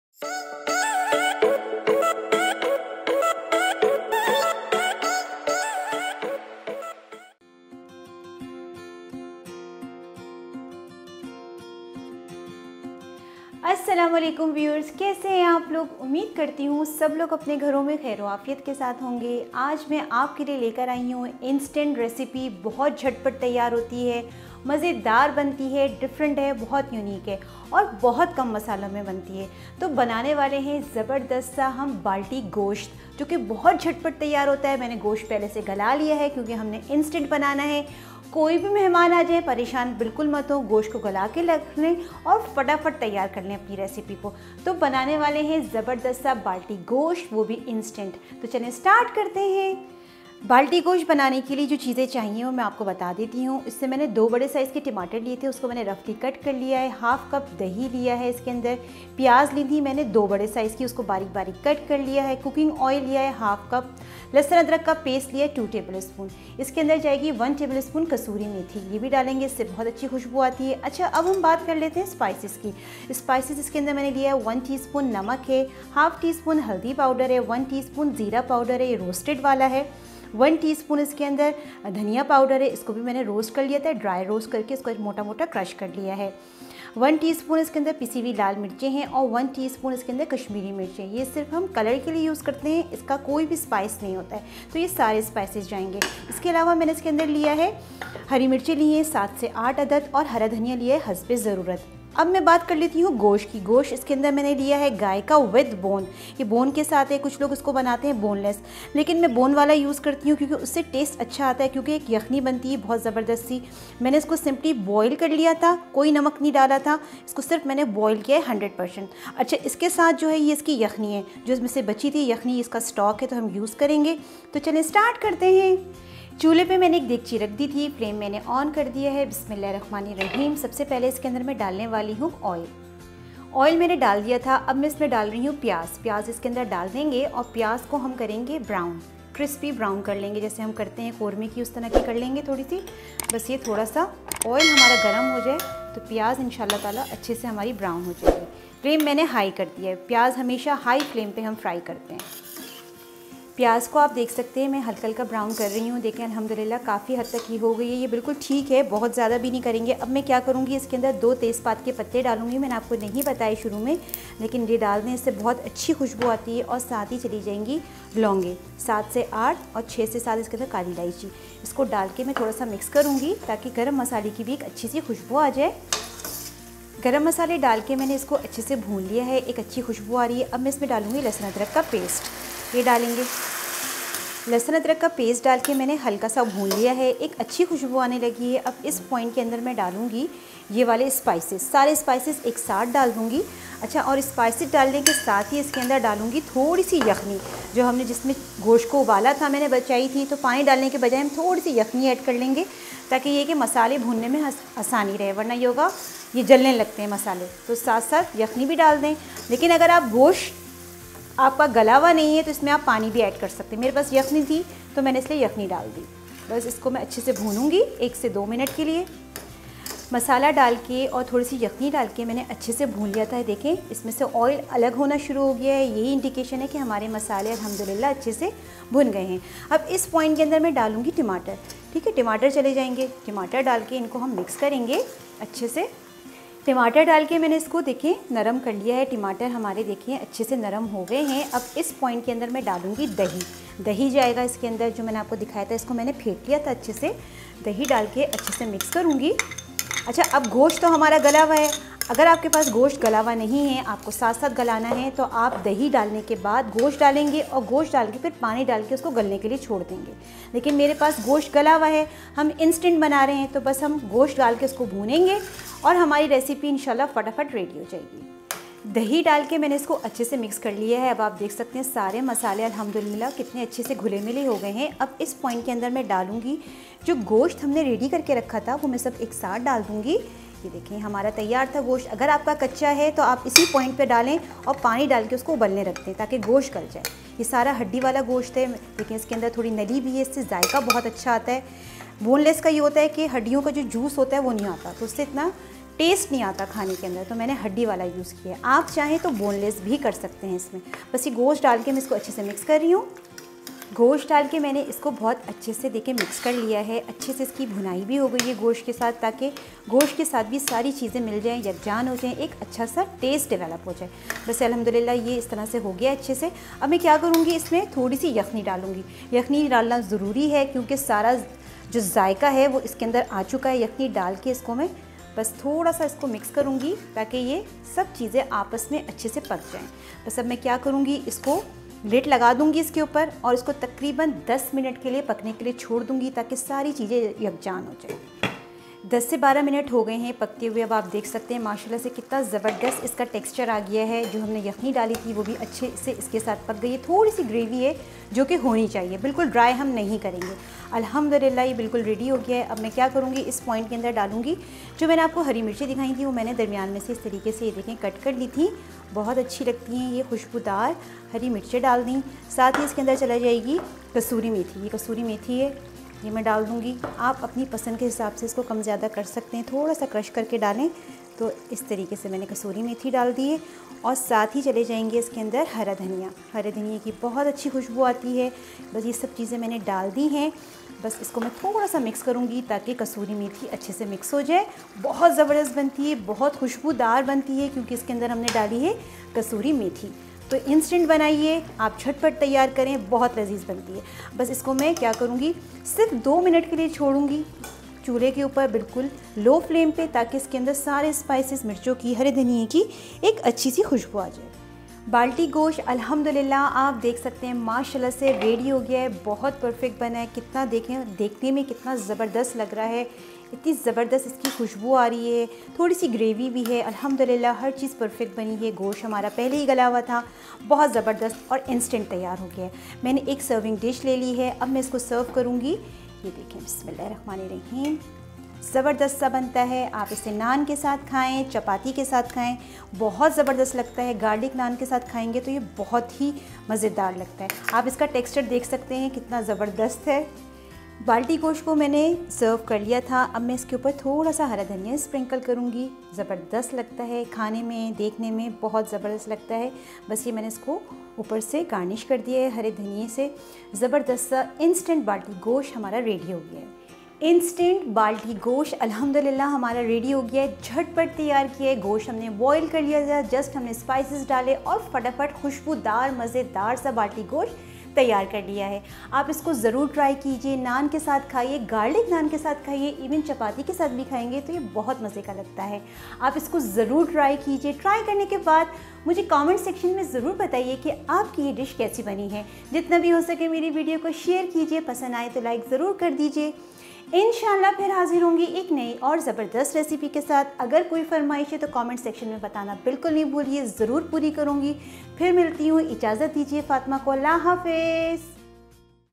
Balti Gosht Recipe by Fatimah in the Kitchen | Restaurant Style Balti Gosht | بالٹی گوشت
How to Make Balti Gosht Recipe. Easy and Tasty Balti Gosht Recipe. Complete Recipe of Balti Gosht Banane ka Tarika. Step by Step Complete Recipe of Balti Gosht by Fatimah in the Kitchen.

Ingredients:

1 Kg Beef with Bone (Boiled)
Beef Stock (Yakhni)
2 Large Tomatoes (Roughly Cut)
½ Cup Yoghurt
2 Large Onions (Finely Cut)
½ Cup Cooking Oil
2 Tbsp Ginger Garlic Paste
1 Tbsp Dried Fenugreek Leaves (Kasuri Methi)
1 Tsp Salt
½ Tsp Turmeric Powder
1 Tsp Cumin Powder (Roasted)
1 Tsp Coriander Powder (Roasted)
1 Tsp Red Chilli Powder
1 Tsp Kashmiri Chilli Powder
7-8 Green Chillies
Coriander as Per Required